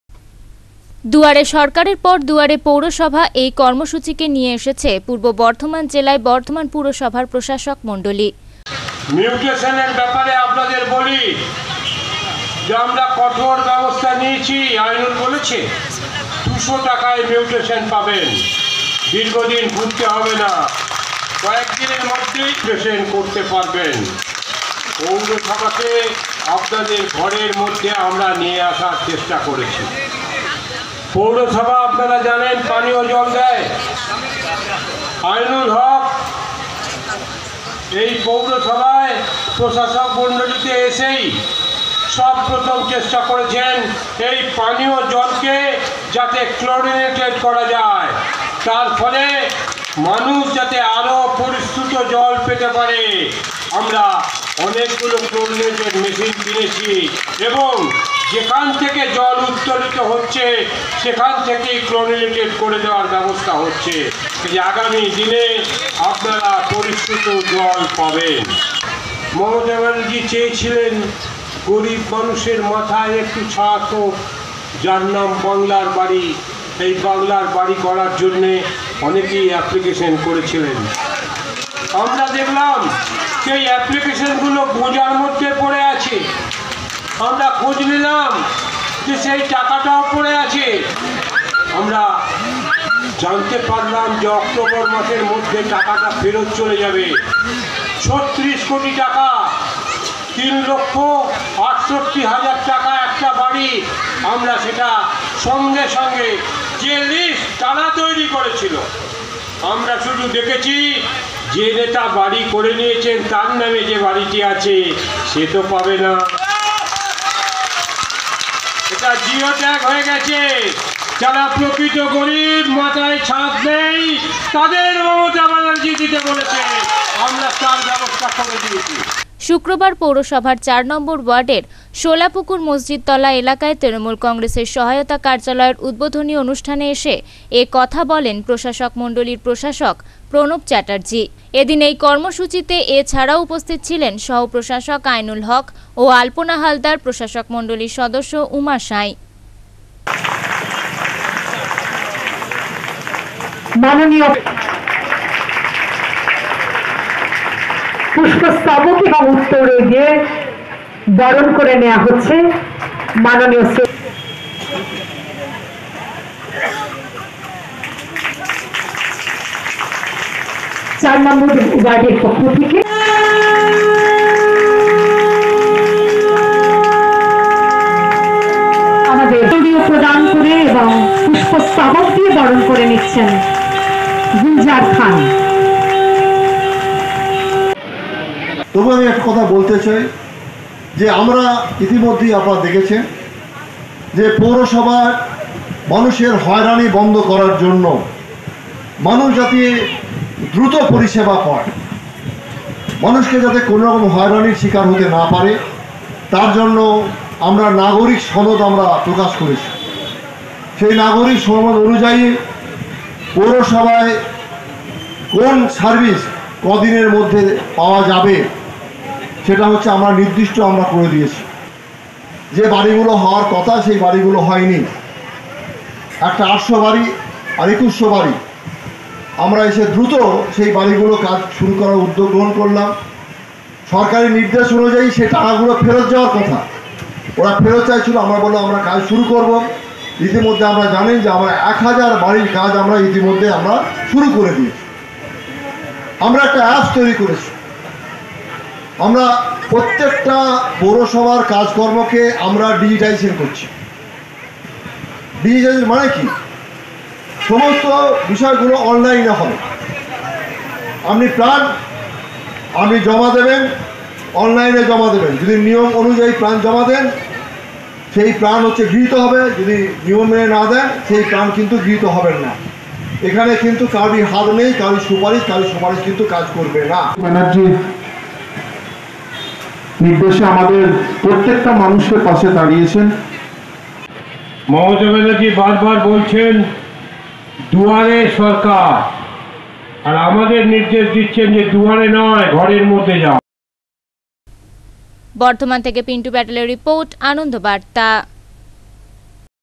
বর্ধমান পৌরসভার প্রশাসক মন্ডলীদের সম্বর্ধনা জানানো হলো
পূর্ব বর্ধমান জেলার শহর বর্ধমানের 4 নম্বর ওয়ার্ড তৃণমূল কংগ্রেসের উদ্যোগে বর্ধমান পৌরসভার প্রশাসক মন্ডলীদের সম্বর্ধনা জানানো হলো